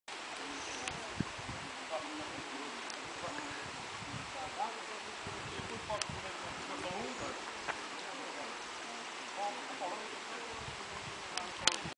Pelos Trilhos de Viriato II - 14
No fim da tarde em Drave, preparativos para a Cozinha Selvagem. Actividade dos Rituais efectuada na Serra da Arada em 12 e 13 de Julho. Caminhada subindo e descendo encostas, tomando banho em rios, acampamento em Drave, cozinha selvagem e fogueira, tudo em excelente convívio